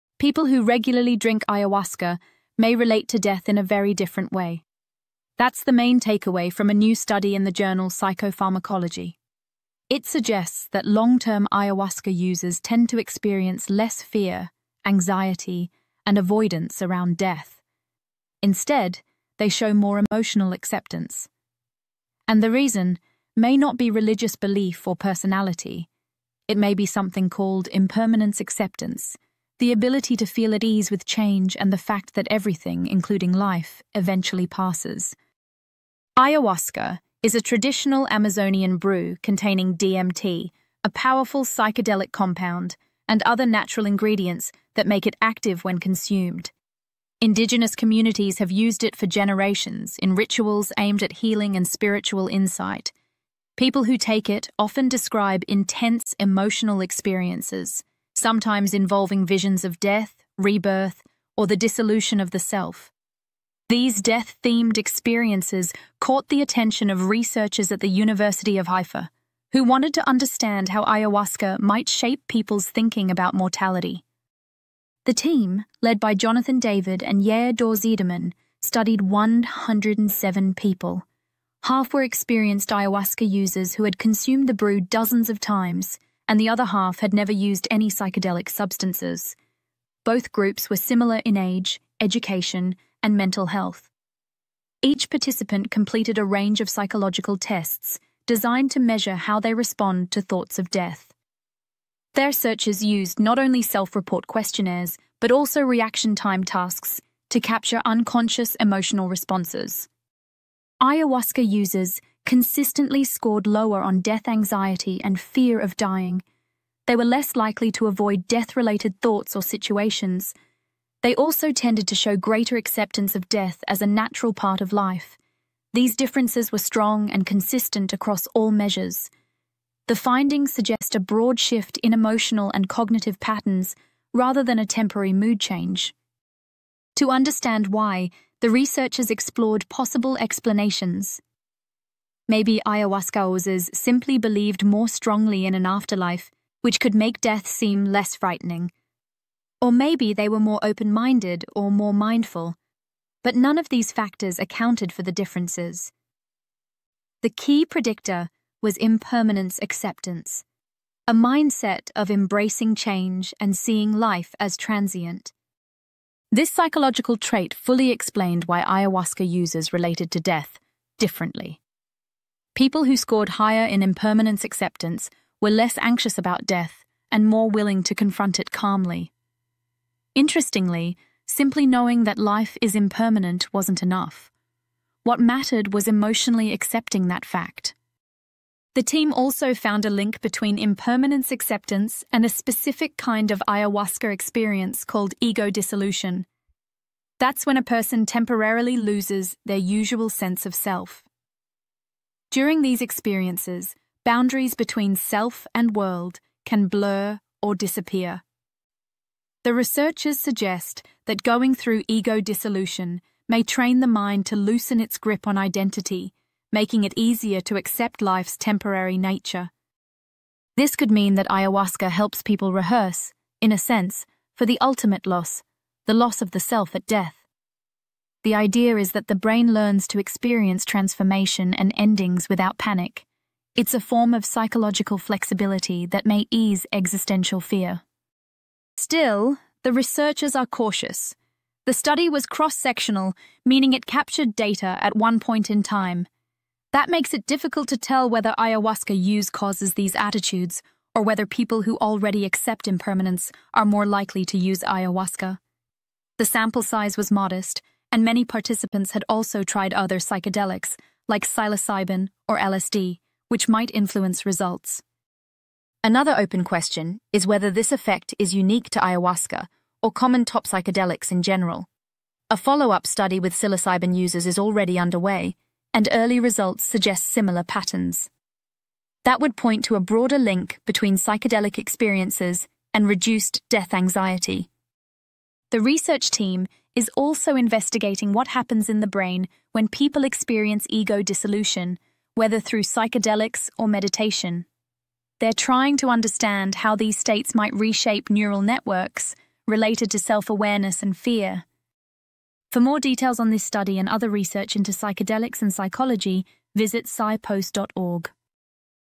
Scientists Explore How Ayahuasca Alters Our View of Mortality
People who regularly use ayahuasca tend to experience less fear and avoidance around death, showing greater emotional acceptance instead. The study suggests this difference is linked not to spiritual beliefs, but to a mindset called impermanence acceptance—the ability to embrace change and the transient nature of life.